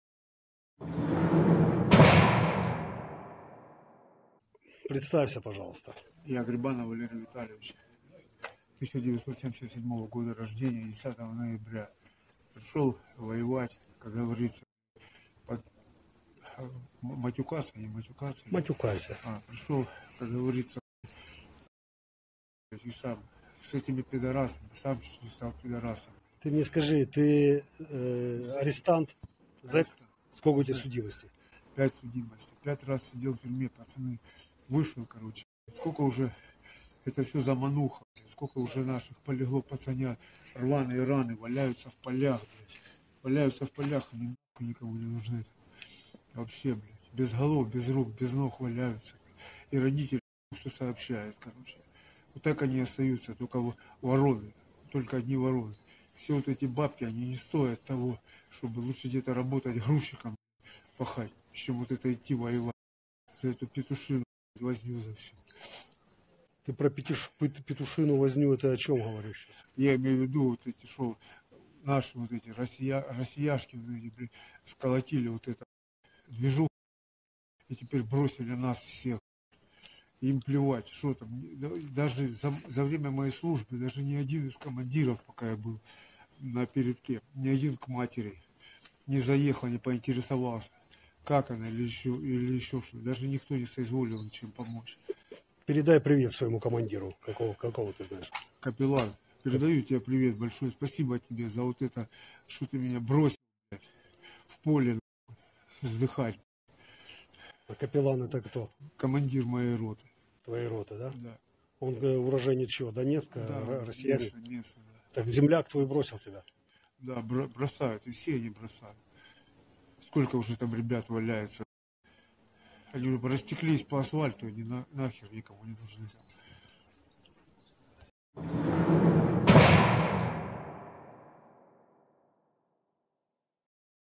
ЭКСКЛЮЗИВ: монолог пленного российского заключённого в Украине
6 июля 2023 года в районе Красногоровки Донецкой области был взят в плен заключённый Грибанов Валерий Витальевич, рядовой 9 роты 3 МСБ 114 ОМСБр 1 АК. Грибанов является гражданином России и Украины, он родился в Донецке, имеет пять судимостей.

Видео оказалось в распоряжении «Руси Сидящей» и MRR.

Bitcoin — 1Pr1vmhRFP6jQ5r87reihtDFyR8pdeqiYT
Ethereum — 0xcCED1A803530ECfD6EaAf75e7C7b5008b48f252A
Litecoin — LRZaRxYSfmBQsvM6fun27Uqu6Gobnywhcs
Ripple — r9e36UdvwMh8v4L9XK7PAE9xLGcf1fG4jZ